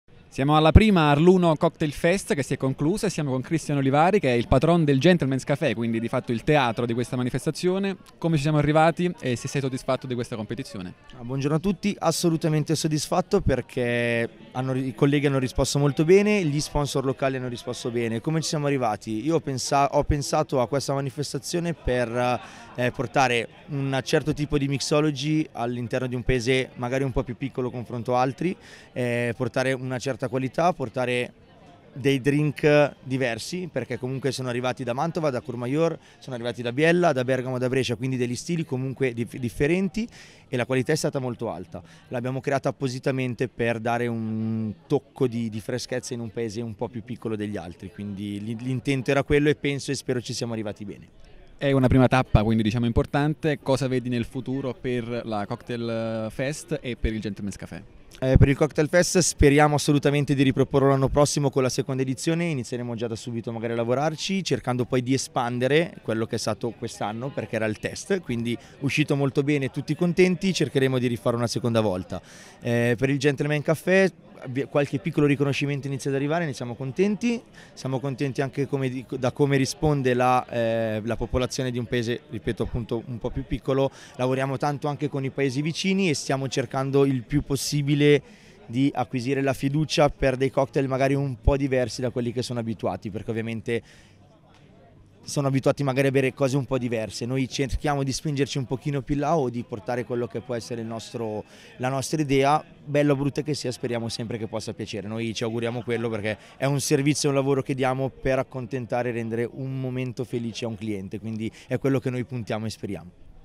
Christian Olivari - Arluno Cocktail Fest 2018
Il commento di Christian Olivari patron del Gentlemen's Cafè, location dove si è svolta la prima edizione del Arluno Cocktail Festival.